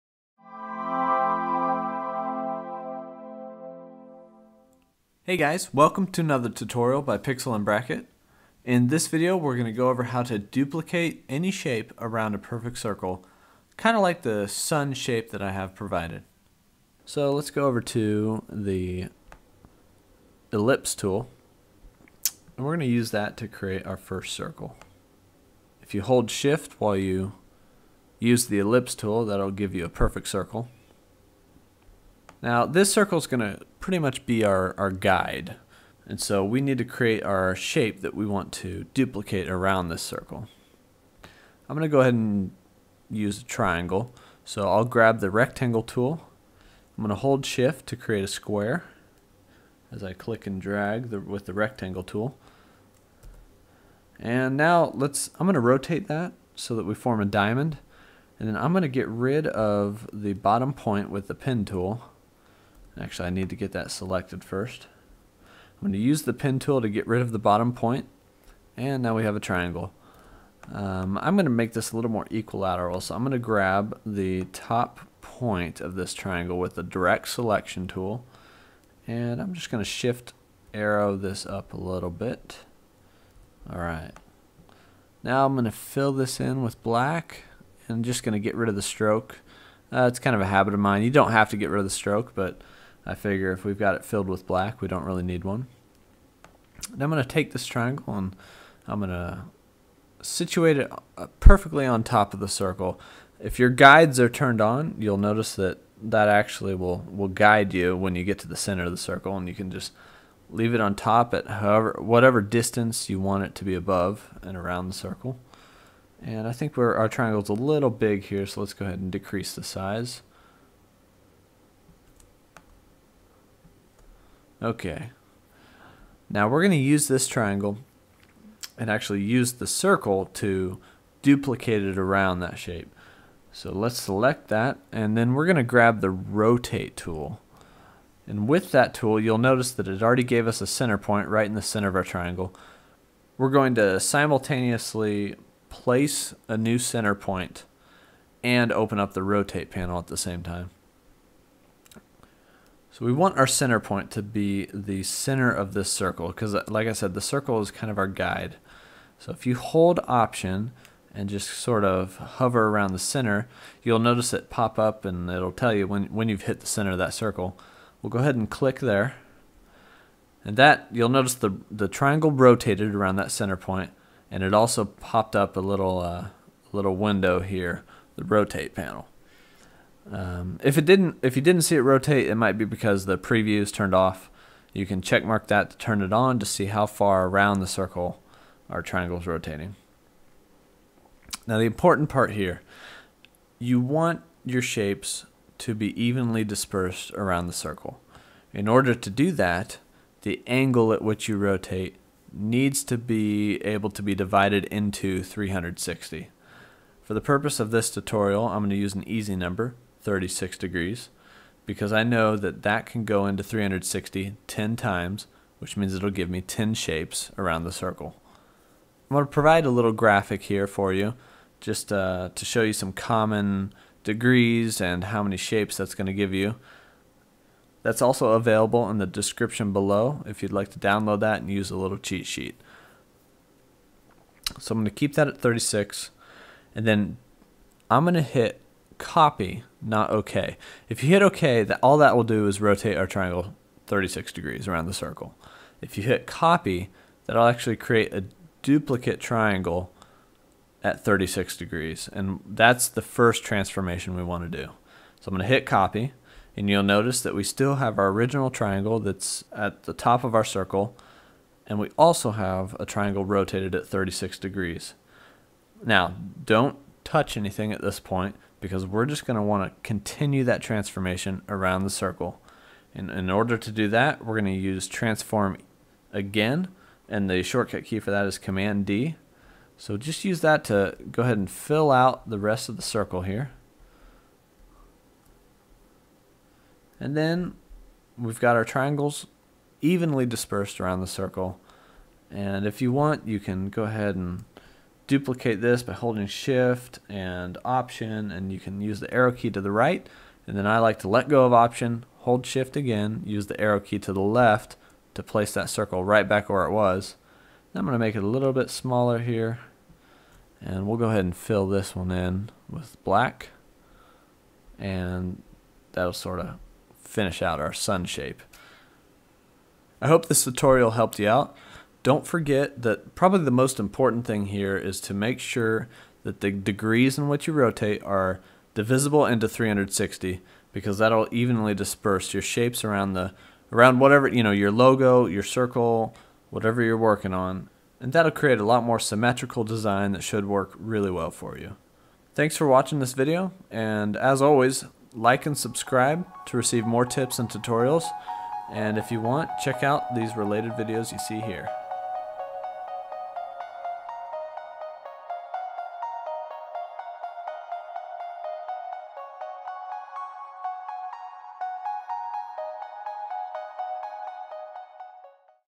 How To Rotate Objects Around a Circle | Illustrator Tutorial
Learn how to rotate objects around a circle by using the Rotate Tool and Transform Again in this Illustrator tutorial.